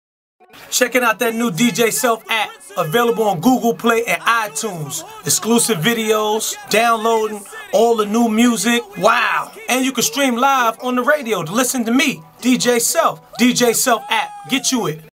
Power 105.1 DJ Self New App Mair Films
Power 105.1 DJ Self Drops His New App For Google Play And iPhones I Make Sure You Visit MyGwinin.com For All Gwinin Gear I Also Download DJ Self New Phone App

Mair Films I Gwinin Films